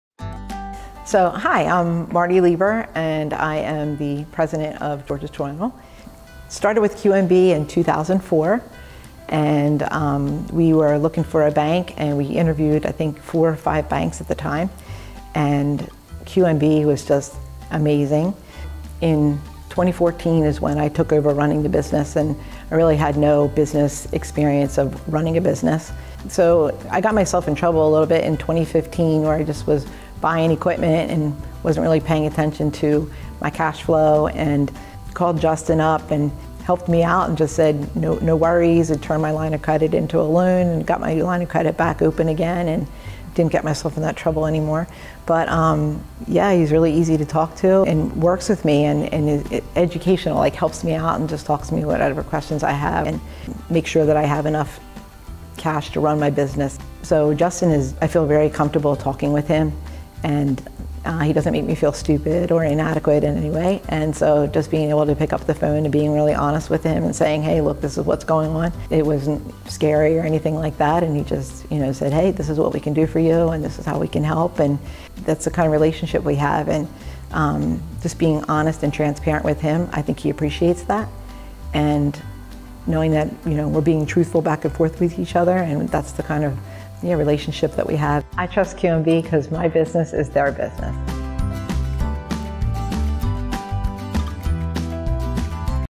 George's Tool Rental - Marty Leber
At QNB, our clients tell the same story. Hear why Marty Leber of George's Tool Rental values her relationship with QNB and says, "My Business is Their Business."